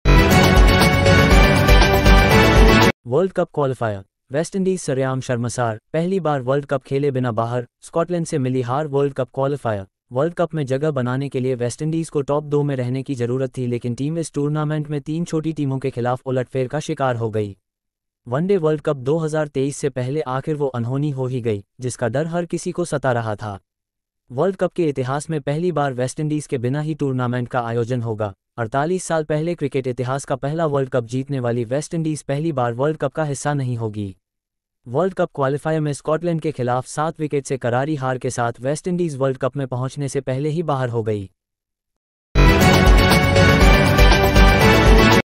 वेस्टइंडीज पहली बार वर्ल्ड कप खेले बिना बाहर
World Cup Qualifier:
 वेस्टइंडीज सरेआम शर्मसार, पहली बार वर्ल्ड कप खेले बिना बाहर, स्कॉटलैंड से मिली हार
World Cup Qualifier:
 वर्ल्ड कप में जगह बनाने के लिए वेस्टइंडीज को टॉप-2 में रहने की जरूरत थी लेकिन टीम इस टूर्नामेंट में तीन छोटी टीमों के खिलाफ उलटफेर का शिकार हो गई.

वनडे वर्ल्ड कप 2023 से पहले आखिर वो अनहोनी हो ही गई, जिसका डर हर किसी को सता रहा था. वर्ल्ड कप के इतिहास में पहली बार वेस्टइंडीज के बिना ही टूर्नामेंट का आयोजन होगा. 48 साल पहले क्रिकेट इतिहास का पहला वर्ल्ड कप जीतने वाली वेस्टइंडीज पहली बार वर्ल्ड कप का हिस्सा नहीं होगी.